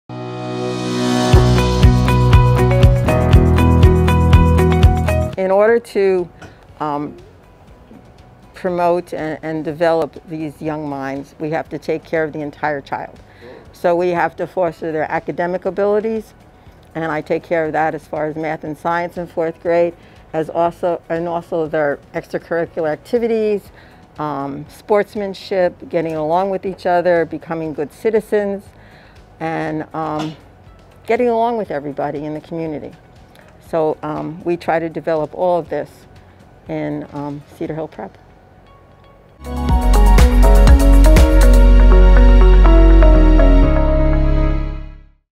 How do we foster development for the whole child? Cedar Hill Prep School
How do we foster development for the whole child? One of our 4th Grade teachers explains the methods for teaching in full at CHP. Learn more now!